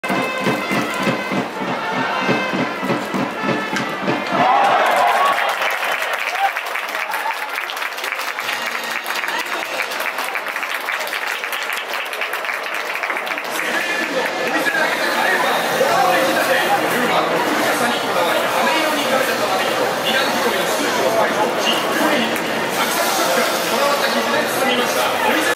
雨の中ファインプレー！強烈なピッチャーライナーに素早く反応する大瀬良大地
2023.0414広島東洋カープvsヤクルトスワローズ